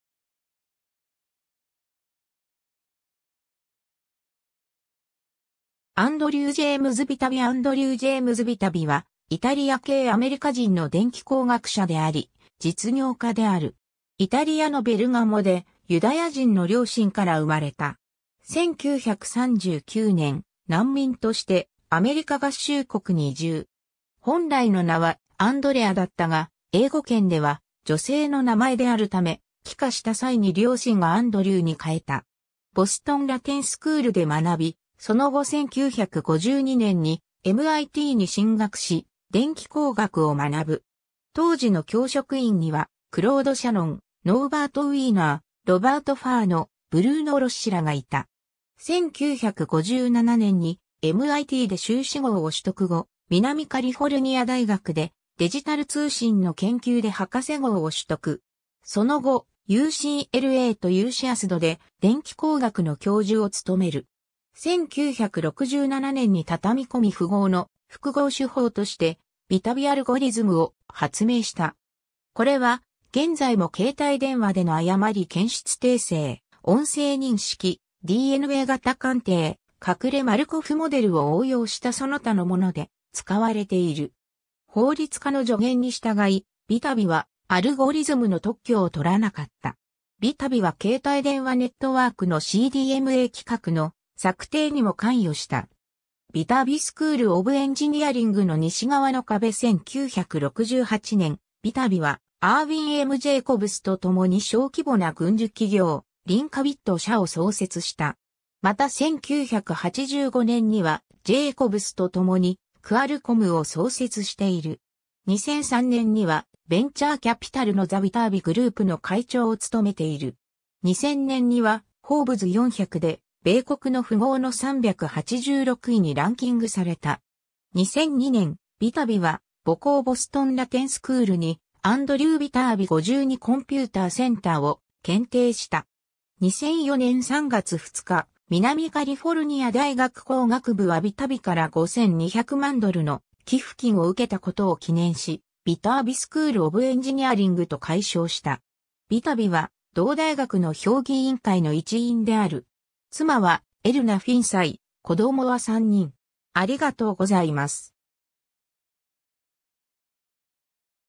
アンドリュー・ビタビ
クロード・E・シャノン賞の受賞者
アンドリュー・ジェームズ・ビタビ アンドリュー・ジェームズ・ビタビ（英: Andrew James Viterbi 、1935年3月9日 - ）は、イタリア系アメリカ人の電気工学者であり、実業家である。
 イタリアのベルガモでユダヤ人の両親から生まれた。
1939年、難民としてアメリカ合衆国に移住。
本来の名はアンドレア（Andrea）だったが、英語圏では女性の名前であるため、帰化した際に両親がアンドリュー（Andrew）に変えた。
Boston Latin School で学び、その後1952年にMITに進学し、電気工学を学ぶ。
当時の教職員には、クロード・シャノン、ノーバート・ウィーナー、ロバート・ファーノ、ブルーノ・ロッシらがいた。
1957年にMITで修士号を取得後、南カリフォルニア大学でデジタル通信の研究で博士号を取得。
 その後、UCLAとUCSDで電気工学の教授を務める。
1967年に畳み込み符号の復号手法としてビタビアルゴリズムを発明した。
これは現在も携帯電話での誤り検出訂正、音声認識、DNA型鑑定、隠れマルコフモデルを応用したその他のもので使われている。
法律家の助言に従い、ビタビはアルゴリズムの特許をとらなかった。
ビタビは携帯電話ネットワークのCDMA規格の策定にも関与した。
 Viterbi School of Engineering の西側の壁 1968年、ビタビはアーウィン・M・ジェイコブスと共に小規模な軍需企業 Linkabit 社を創設した。
また1985年には Jacobs と共にクアルコムを創設している。
2003年には、ベンチャーキャピタルの The Viterbi Group の会長を務めている。
2000年には、フォーブズ400で米国の富豪の386位にランキングされた（推定資産は6億4000万ドル）。
 2002年、ビタビは母校 Boston Latin School に Andrew Viterbi '52 Computer Center を献呈した。
2004年3月2日、南カリフォルニア大学工学部（School of Engineering）はビタビから5200万ドルの寄付金を受けたことを記念し、Viterbi School of Engineering と改称した。
ビタビは同大学の評議委員会の一員である。
 妻は Erna Finci、子供は3人。